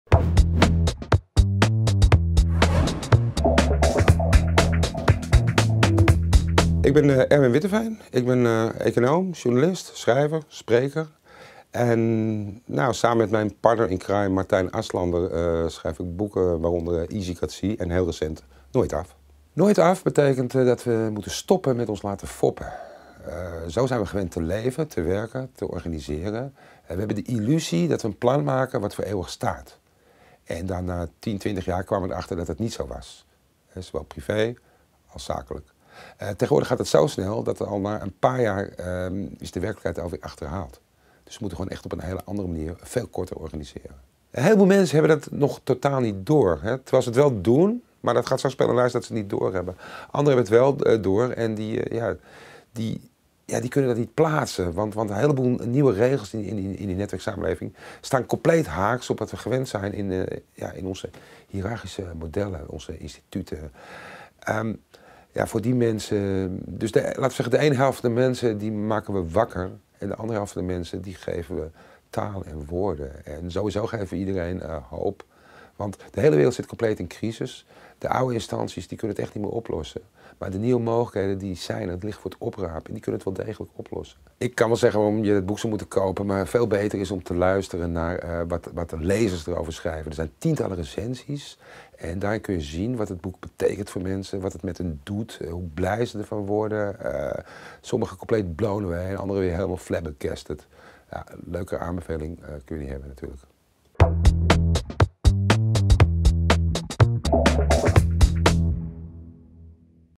Erwin Witteveen over Nooit Af
Nieuwe technologieën, vormen van samenwerken en verdienmodellen: de kranten staan er bol van. De veranderingen gaan nu zo snel dat onze organisaties en werkvormen, die nog gericht zijn op het vinden van langdurig werkende oplossingen, steeds vaker vastlopen. In Easycratie, het vorige boek van Martijn Aslander en Erwin Witteveen, beschreven ze wat deze nieuwe netwerk- en informatiesamenleving inhoudt en hoe deze is ontstaan, nu ligt de focus op de gevolgen ervan.

'Nooit Af' gaat over wat er gebeurt nu iedereen vrij kan publiceren, dupliceren en produceren en wat de impact is op de maatschappij en economie als mensen dit niet alleen om financiële redenen doen, maar ook om een steentje bij te dragen aan een betere wereld. Dit levert ongekende mogelijkheden op die bovendien voortdurend groter en beter worden. Er is helemaal geen tijd meer voor blijvende oplossingen.